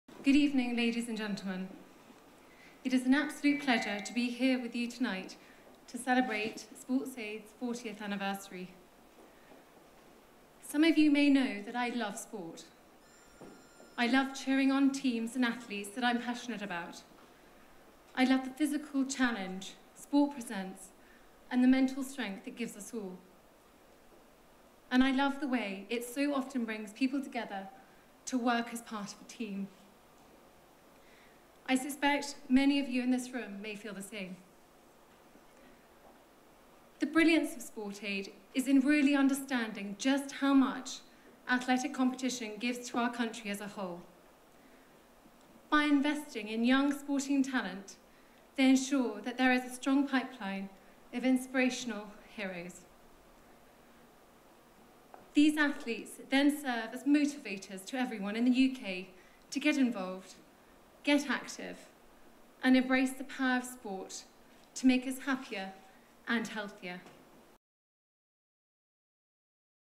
Kate attends dinner to celebrate 40 years of SportsAid
The Duchess of Cambridge talks about her love of sport and why it's so important for young people as she attended SportsAid's 40th Anniversary dinner.